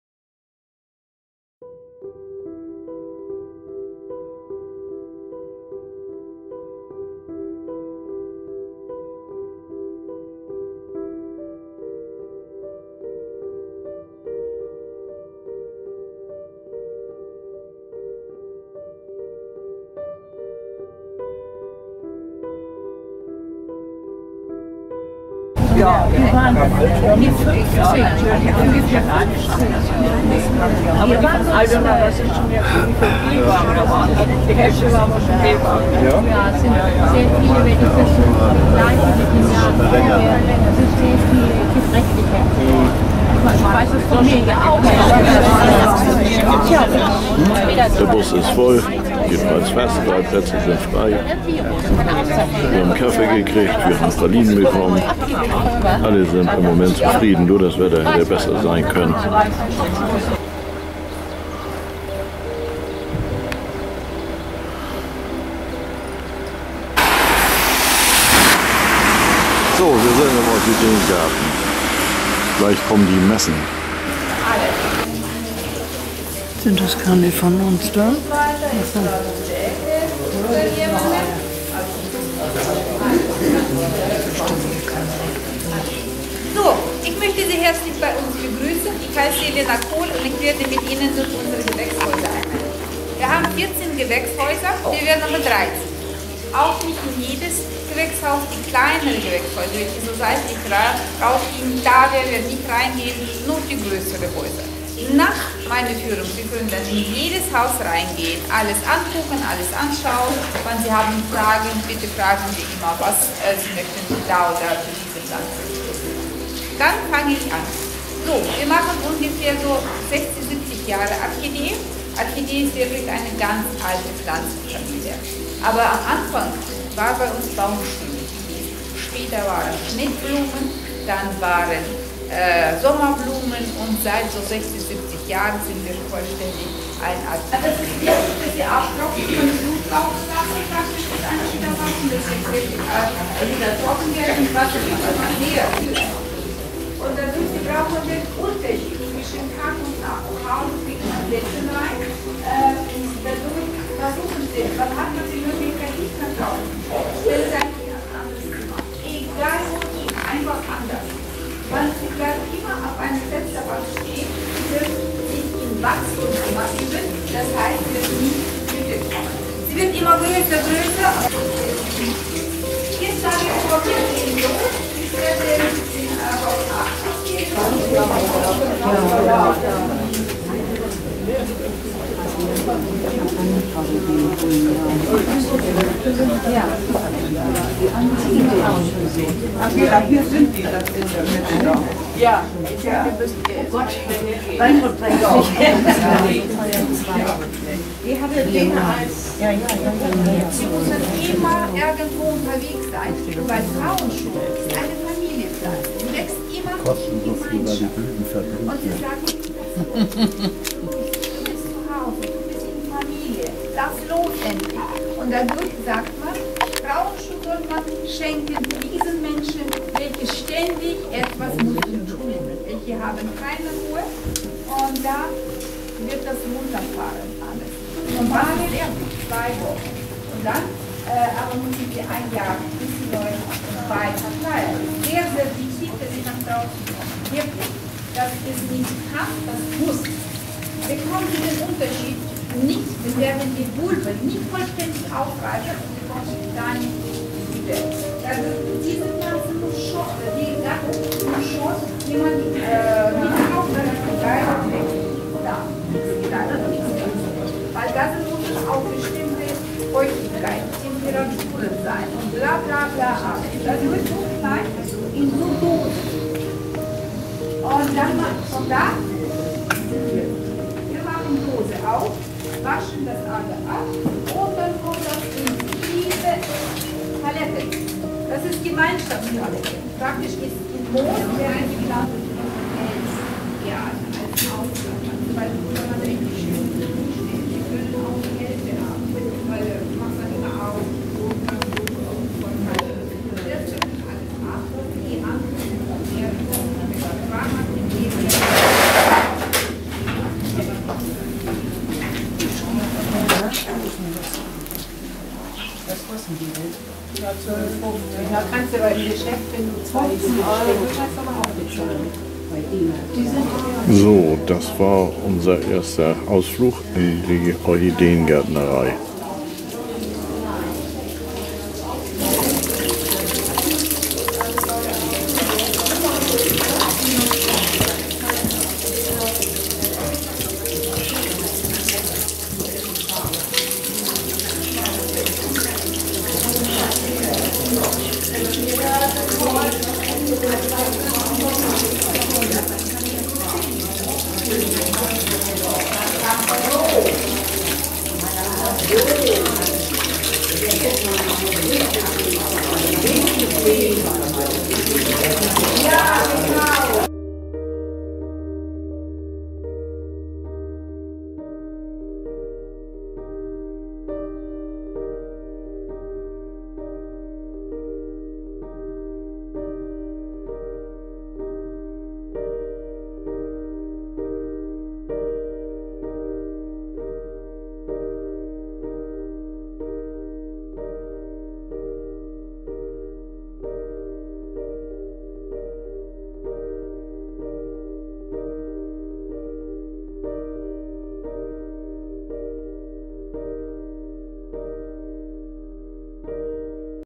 Mein Film Orchideenfahrt am 14 10 2015
Am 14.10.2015 fuhren die beiden Freizeitgruppen Elbvororte und Freizeitgruppe Süderelbe nach Dahlenburg um die Orchideengärtnerei zu besuchen, anschl. ging es weiter in das Landhaus Karze wo die Teilnehmer ein Gans-Büffet erwartete, es gab viele Köstlichkeiten von der Gans und alle Teilnehmer waren sehr zufrieden.